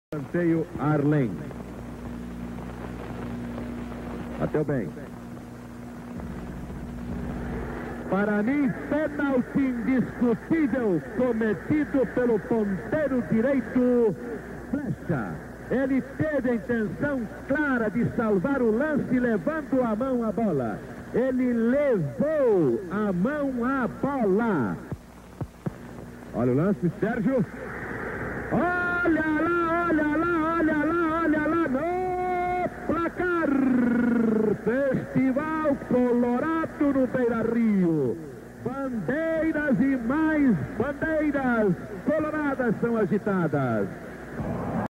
17/10/1971 - Internacional 1x0 Grêmio - Campeonato Brasileiro 1971
17/10/1971

Internacional 1x0 Grêmio - Campeonato Brasileiro 1971

Gol do Inter: Sérgio Galocha